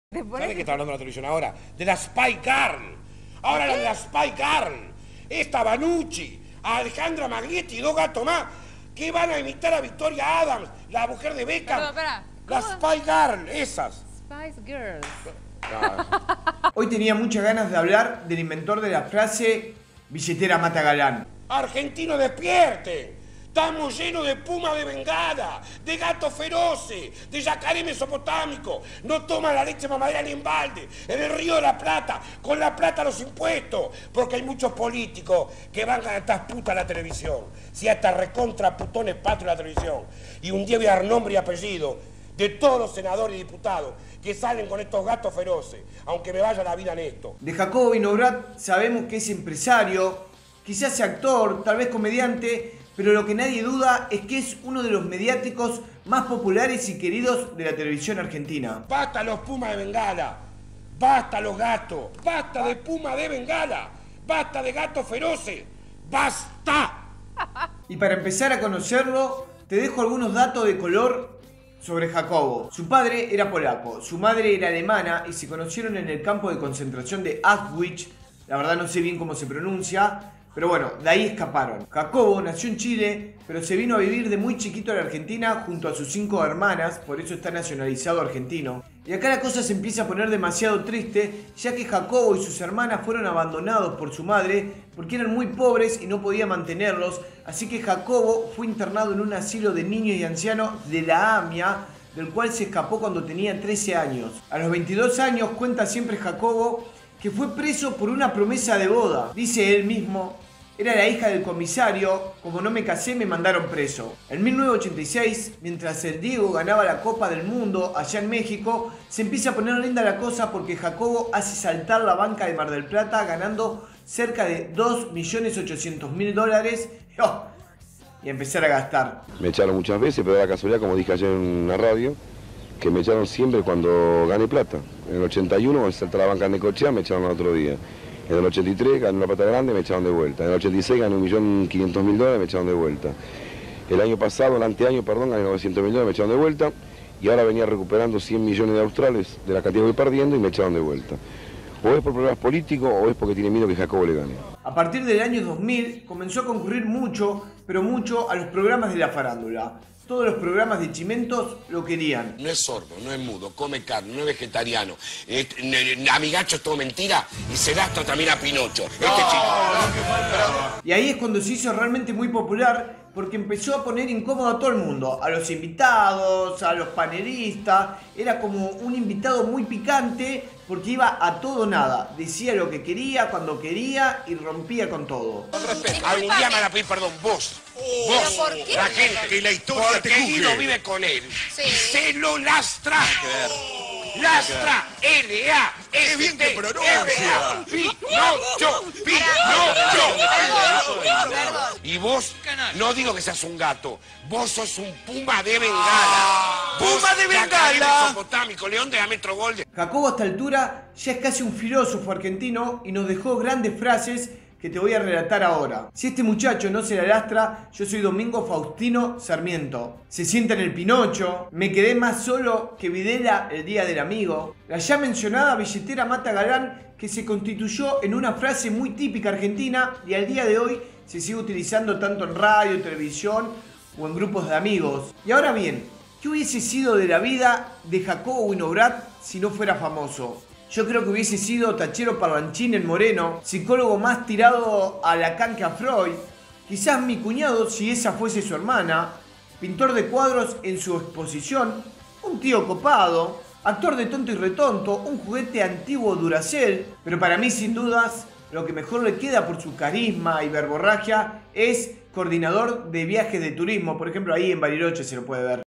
¿Que hubiese sido de la vida de Jacobo Winograd si no fuese famoso? Hizo saltar la banca del casino!
jacobo winobrat sabemos que es empresario, ¿quizás  actor?, ¿ tal vez comediante?

Pero lo que nadie duda, es que es uno de los mediaticos más populares y queridos de la tv argentina

te dejo algunos datos de color sobre jacobo:

- Su padre era polaco y su madre alemana, y se conocieron en el campo de concentración de Auschwitz, del que escaparon.

- Jacobo nació en Chile pero se vino a vivir de muy niño a Bs As junto a sus 5 hermanas.

- Su madre los abandonó debido a que eran muy pobres y jacobo fue internado en un asilo de niños y ancianos de la AMIA del cual se escapó a los 13 años.

-  Siempre cuenta que a 22 los años, estuvo preso un mes por promesa de boda:
Era la hija del comisario. Como no me casé me mandaron preso.

- En 1986 hizo saltar la banca del Casino de Mar del Plata, ganando cerca de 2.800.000 dólares y acá se empieza a poner linda la cosa.

- A partir de 2000 comenzó a concurrir a programas de televisión que se basaban en la vida de la farándula (programas de chimento),

 Allí se hizo muy popular como invitado picante, que ponía incómodo a los demás invitados o bien a los panelistas del programa en cuestión, por sus comentarios a todo o nada.

Es el inventor de las frases:

Si este muchacho no se la lastra, yo soy Domingo Faustino Sarmiento.

Se sienta en el pinocho.

Me quedé más sólo que Videla en el día del amigo.

Billetera mata galán. frase, que se ha constituido en su más importante cita y actualmente se la utiliza frecuentemente en medios como la televión y la radio.

Yo casé más gente que Roberto Galán.

Estamos llenos de pumas de Bengala! De gatos feroces! De yacarés mesopotámicos! en lugar de yaguaretés mesopotámicos

Ahora: que hubiera sido de la vida de Jacobo si no fuera famoso:

tachero parlanchín en Moreno

psicologo mas tirado a lacan que a freud

quizás mi cuñado si esa fuese la hermana
Pintor de cuadros en su exposición

un tío copado

actor de tonto y retonto

un juguete antiguo duracell

Pero para mi sin duda lo que mejor le queda por su carisma y verborragia es coordinador de viajes de turismo.